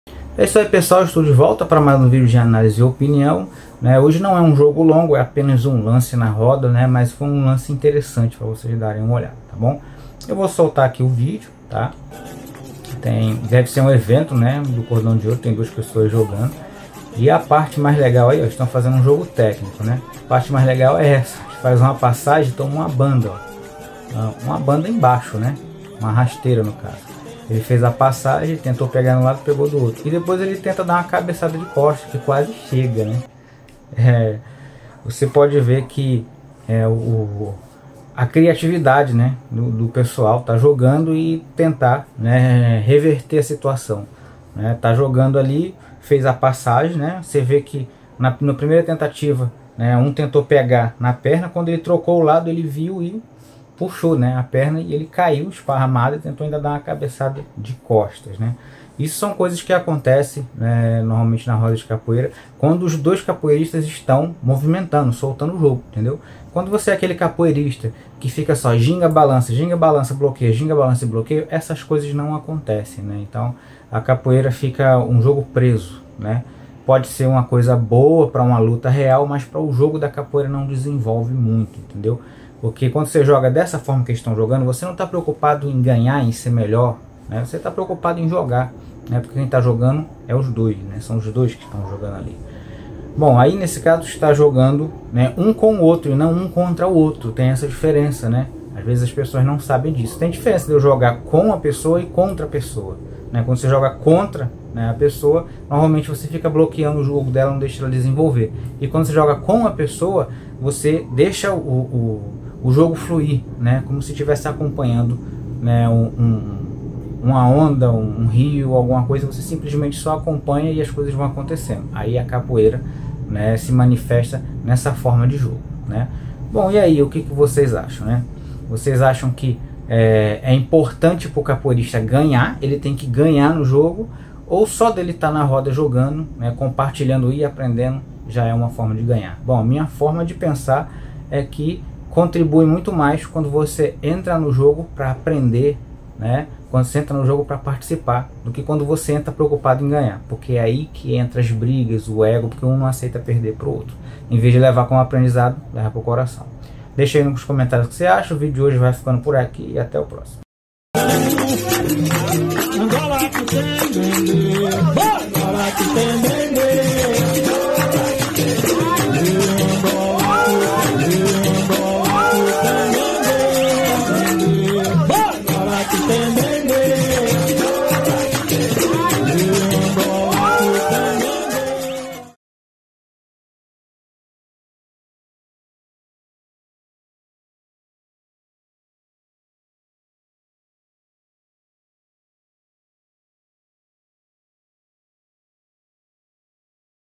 Coisas assim só acontece na capoeira - No jogo de capoeira tem que haver um vencedor?
CANAIS QUE RECOMENDO
VRT INFO TEC: